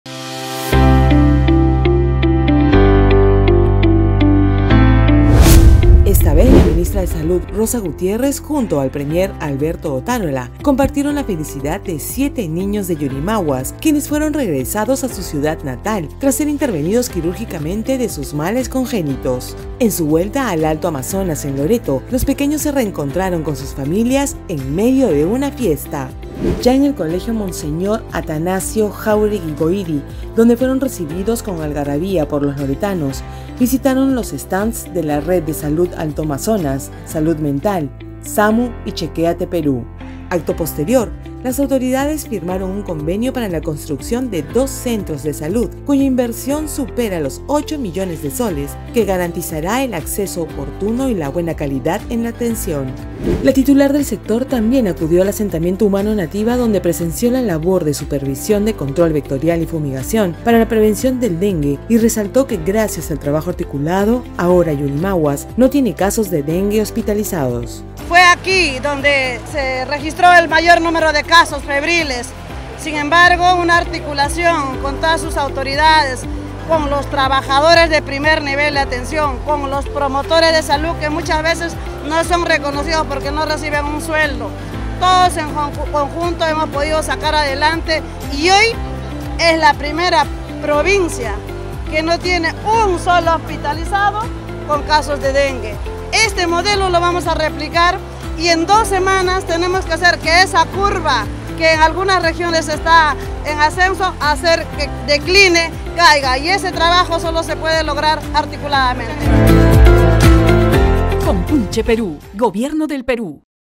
𝗬𝘂𝗿𝗶𝗺𝗮𝗴𝘂𝗮𝘀 𝘀𝗲 𝗵𝗮 𝗰𝗼𝗻𝘃𝗲𝗿𝘁𝗶𝗱𝗼 𝗲𝗻 𝗲𝗹 𝗽𝗿𝗶𝗺𝗲𝗿 𝗱𝗶𝘀𝘁𝗿𝗶𝘁𝗼 𝗾𝘂𝗲 𝗻𝗼 𝗿𝗲𝗴𝗶𝘀𝘁𝗿𝗮 𝗰𝗮𝘀𝗼𝘀 𝗵𝗼𝘀𝗽𝗶𝘁𝗮𝗹𝗶𝘇𝗮𝗱𝗼𝘀 𝗽𝗼𝗿
"Operación Vida" sigue recuperando sonrisas, regresan 7 nIños a Yurimaguas operados exitosamente.